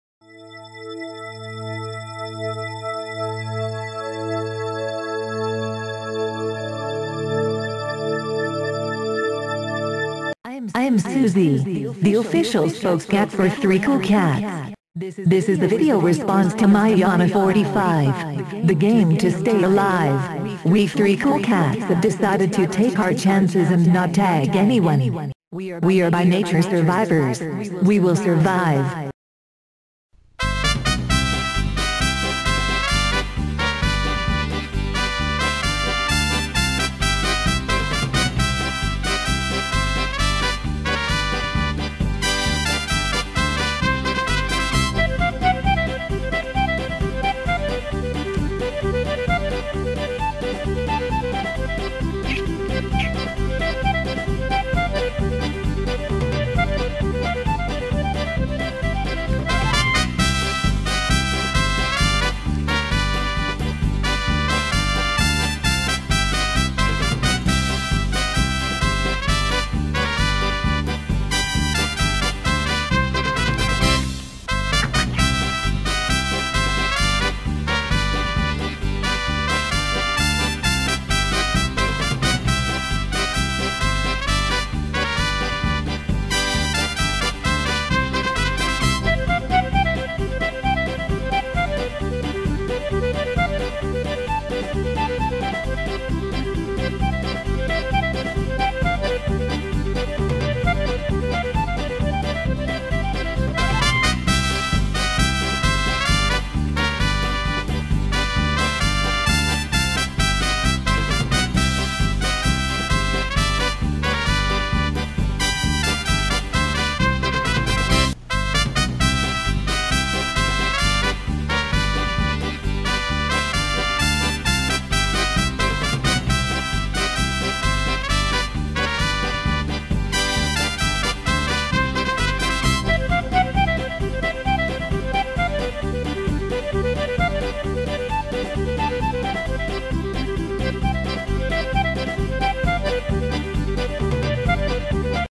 3CoolKats, Episode 42, RE: Let the game begin
A video response to Maiyana45. This will make the 3rd video response she's recieved in the game to stay alive begun by GAMS1981, and thus completing her requirement to get 3 responses. 3CoolKats have chosen to live on the wild side and tempt fate by not tagging 3 more people. We are more powerful than the evil forces lurking about that might smite us down!